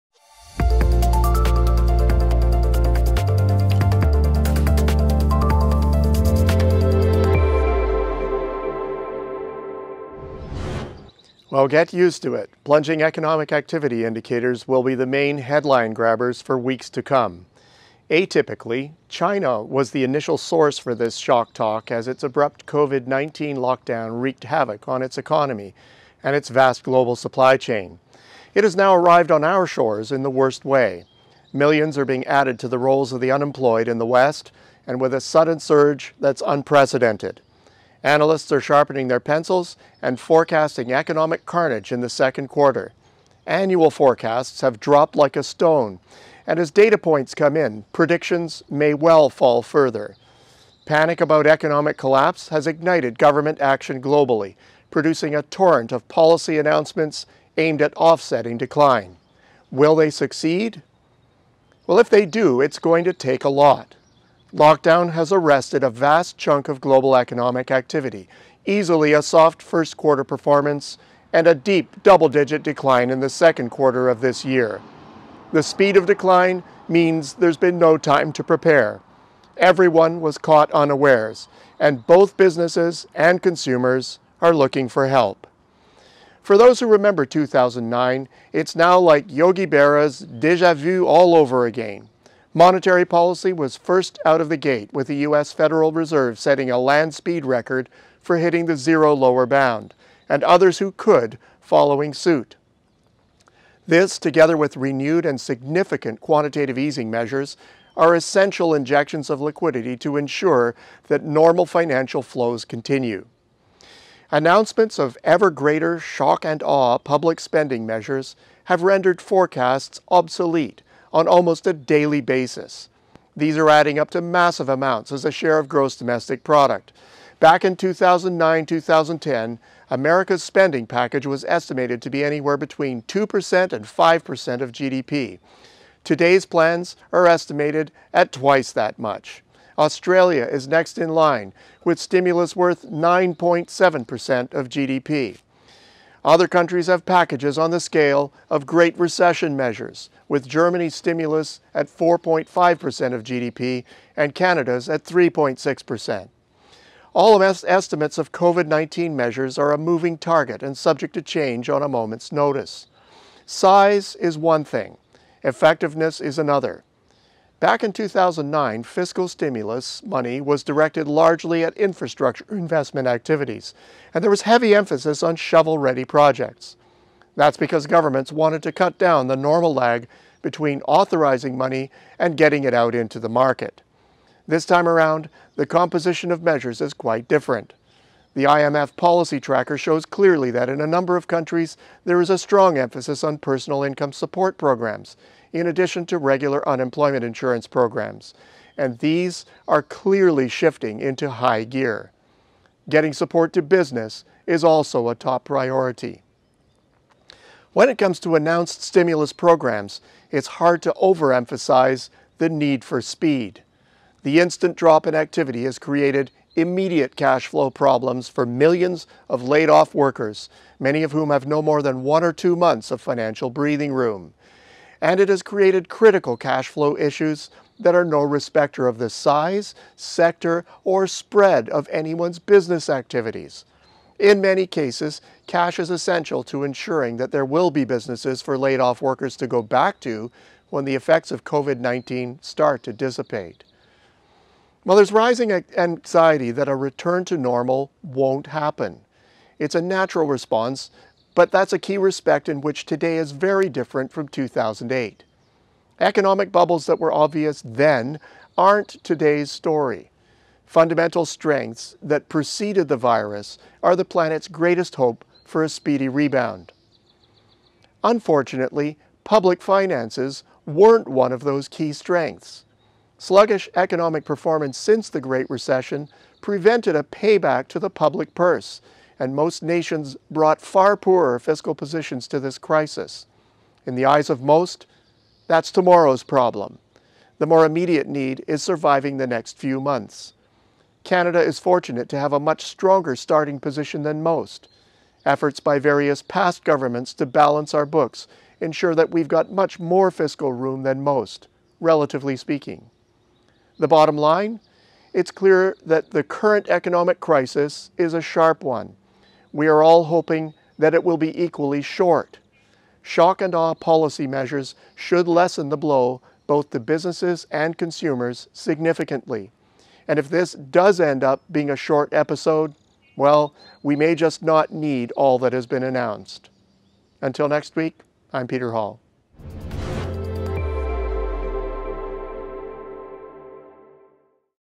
The coronavirus policy response
In global government stimulus programs, there’s an urgent need for speed.